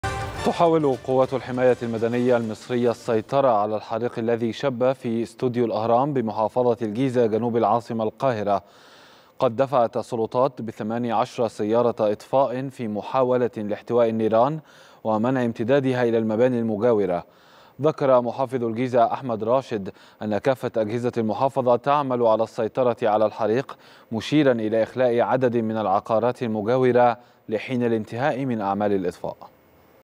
قوات الحماية المدنية المصرية تحاول السيطرة على حريق شب في ستوديو الأهرام بمحافظة الجيزة
16/3/2024